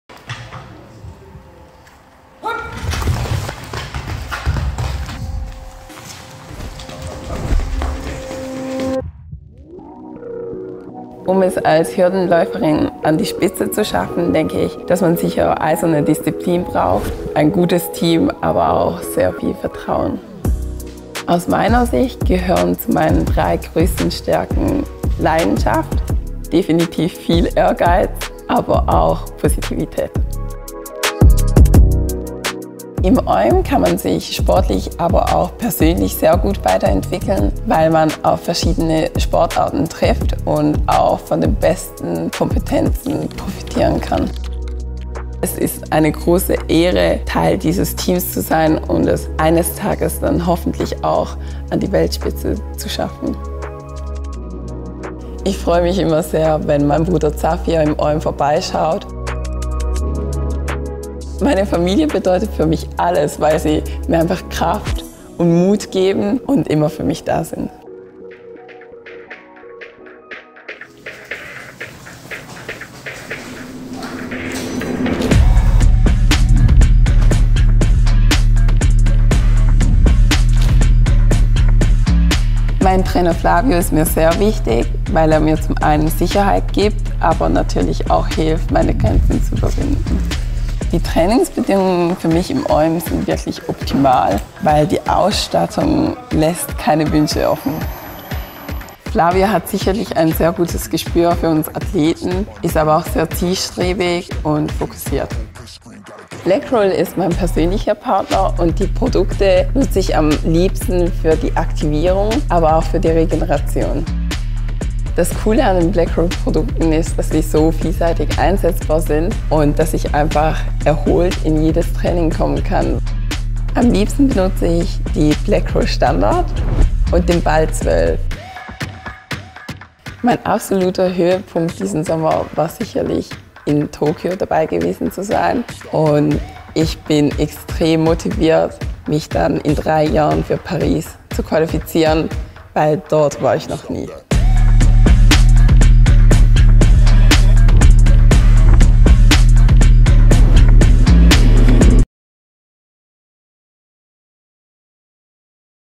BLACKROLL PORTRAIT | Yasmin Giger
May we introduce? She is fast & she wants to become even faster.🏃🏾‍♀️ The Swiss track & field athlete Yasmin Giger.

We were allowed to accompany Yasmin on a training day & take you into the everyday life of the professional athlete.👇

3:25,90! The moment this time lights up on the giant scoreboard in the stadium in Tokyo, the spectators in the stadium and on TV hear loud cries of joy - from the team of the Swiss 4×400 m women's relay. New Swiss record! The old record from 1993, beaten by almost 3 seconds. The ladies immediately realise that this great time is not enough to make it into the final run, but that doesn't matter to them at this moment. They are the fastest Swiss women. This time on the scoreboard is confirmation that they are on the right track to make it to the top of the world. The final runner at this race in Japan is the young runner Yasmin Giger.🤩

An incredible success for the strong Swiss women's relay team and for Yasmin. She has already achieved a lot in her young career - 6 medals on international stages, including a gold medal at the U20 European Championships.🥇 Her next big goal: the 2024 Olympic Games in Paris.

Whether it's 400m runs or the 400m hurdles, Yasmin has mastered both disciplines. Sprinting the long 400m, with or without hurdles, in relays or individual starts - Yasmin is very fast and wants to become even faster.
To achieve this, she trains every day at what is probably one of the most innovative training centres in Europe, the OYM in Cham. There she completes a business training course at the same time.📚

With all the focus on competitive sports, her training, travelling to competitions and public appointments, it is enormously important for Yasmin to keep her natural positivity and joy in sports. Hardly a moment goes by in the training centre when Yasmin is not laughing. For her, the fun is only over when it's time to start and she sees the running track in front of her. Then Yasmin shows her talent and focuses from one moment to the next.💯

Yasmin's goal is to laugh under the Eiffel Tower in 2024 - preferably with a new Swiss record in her pocket. And as the saying goes: the journey is the destination. For Yasmin, however, fun, joy & looseness must never be lost along the way. That's why she loves to use the fascia ball or the fascia roller.

Thank you, dear Yasmin for the insight into your daily training routine & all the best for you and your future.🥰🤩🤗

BLACKROLL® supports Yasmin - BLACKROLL® supports everybody.

⚫️ BLACKROLL® INFORMATION ⚫️